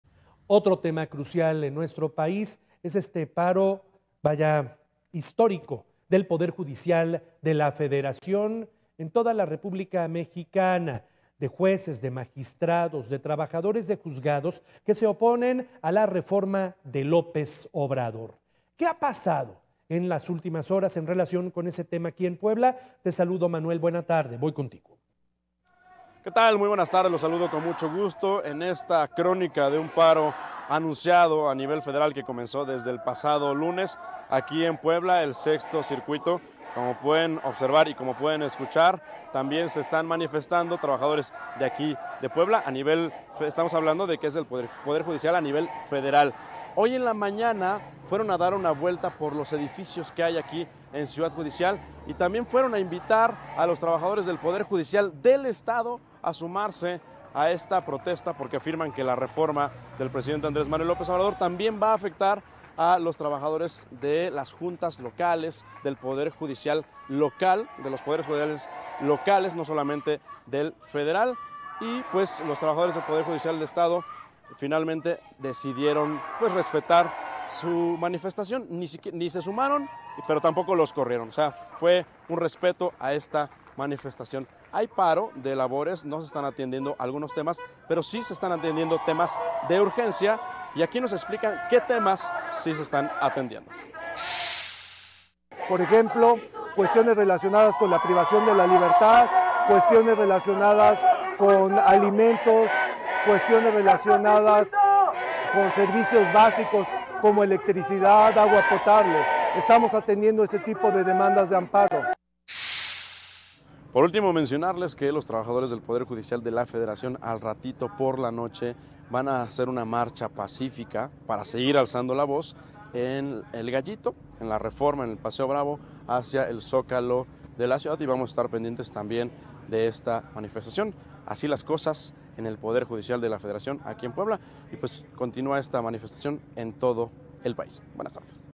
Manifestación Poder Judicial || Noticias con Juan Carlos Valerio
Importante || El Sindicato Independiente de Trabajadores de Volkswagen, publica el nuevo preacuerdo que se tiene con la armadora. Destaca un aumento global del 10.59%.💰✅